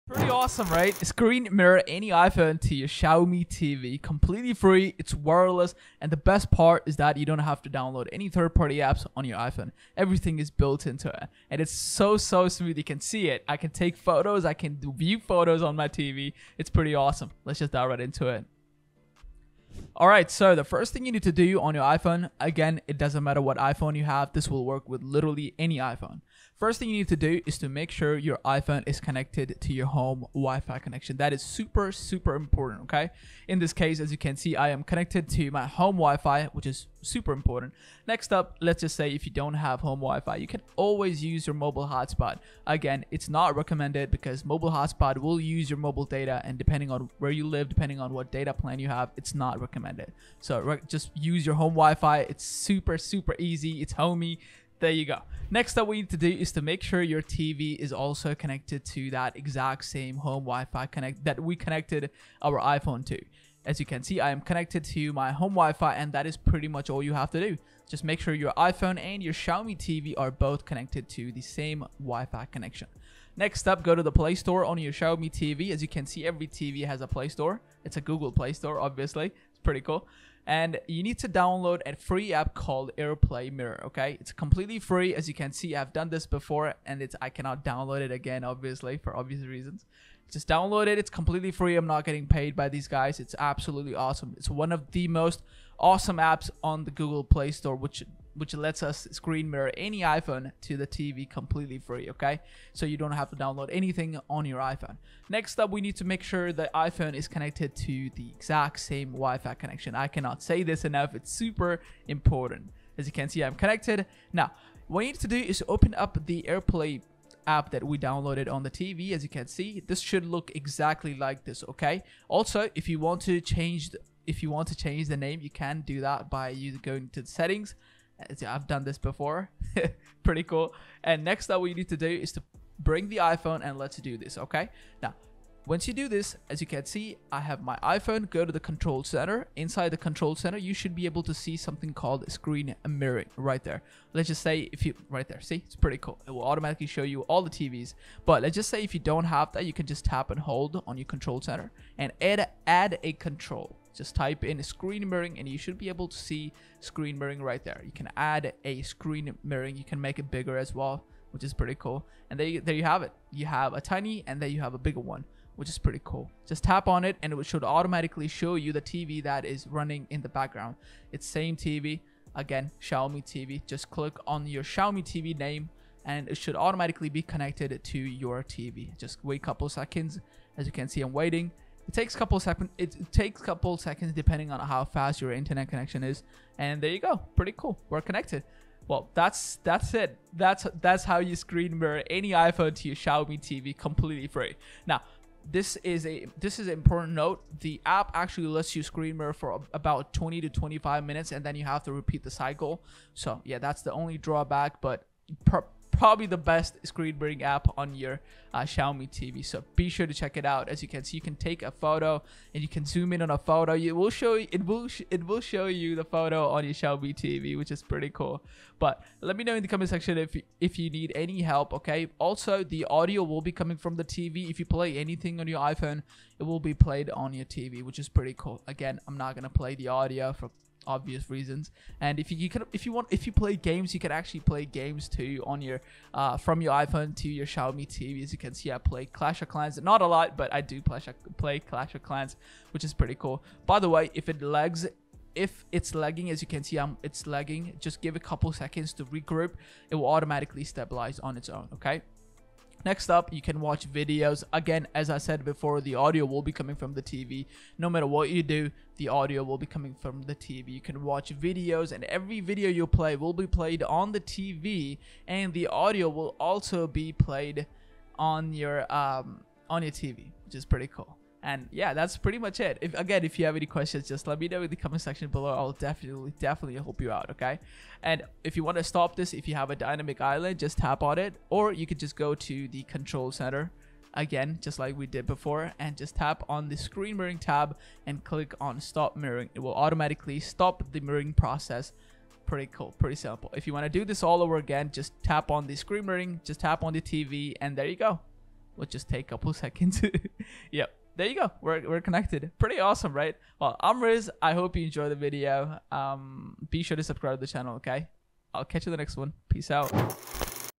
How to Screen Mirror iPhone to Xiaomi Mi TV (Free & Wireless)
How to Screen Mirror iPhone to Xiaomi Mi TV - Free and Wireless

Let me show you How to Screen Mirror any iPhone to any Xiaomi Mi TV completely free and wireless. Without Downloading any 3rd-Party Apps on your iPhone. As Screen Mirroring Feature is built-in iPhones.